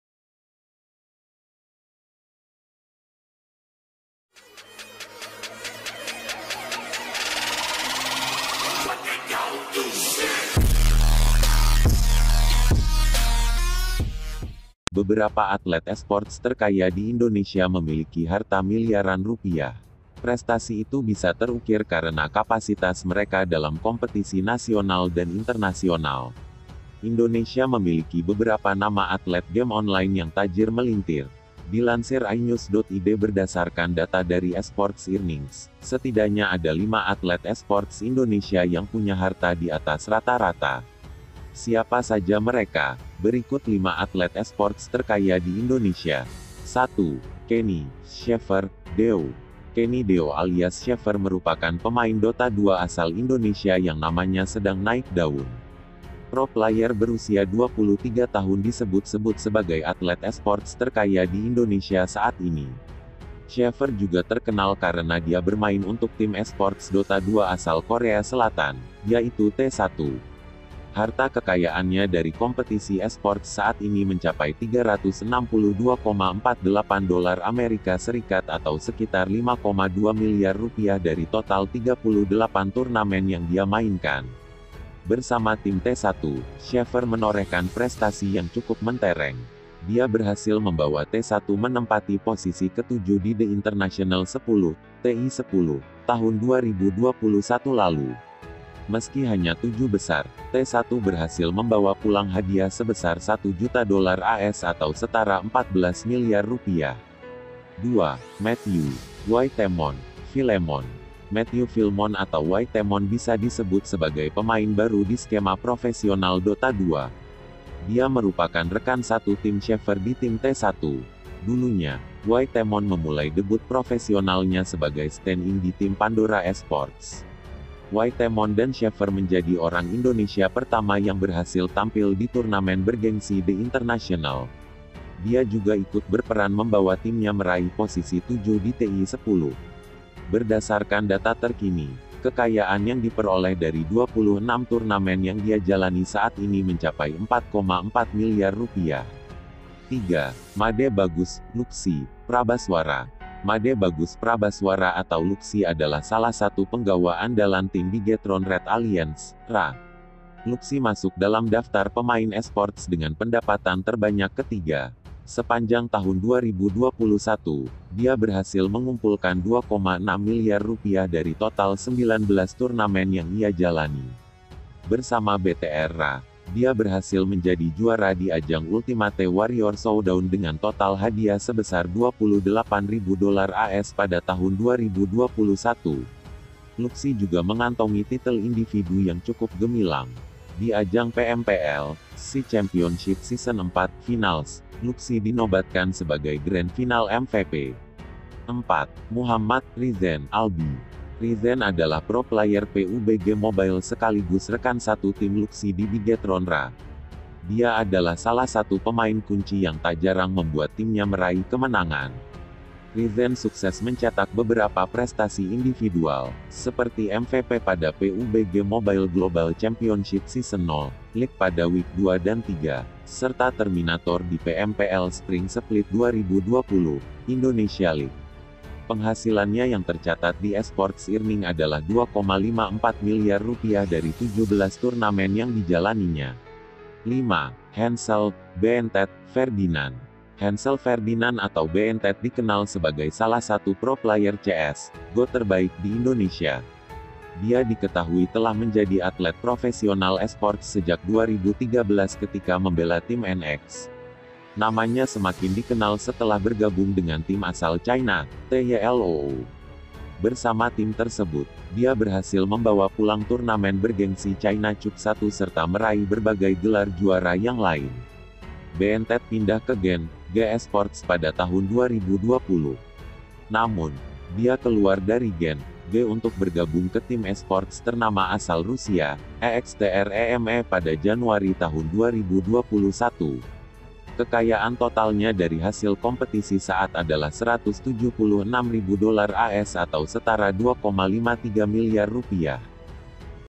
Berita Pro Player, 5 Atlet Esport Terkaya Di Indonesia, Siapa Paling Tajir?
Berita Esport, 5 Atlet Esport Terkaya Di Indonesia, Siapa Paling Tajir?

•Thanks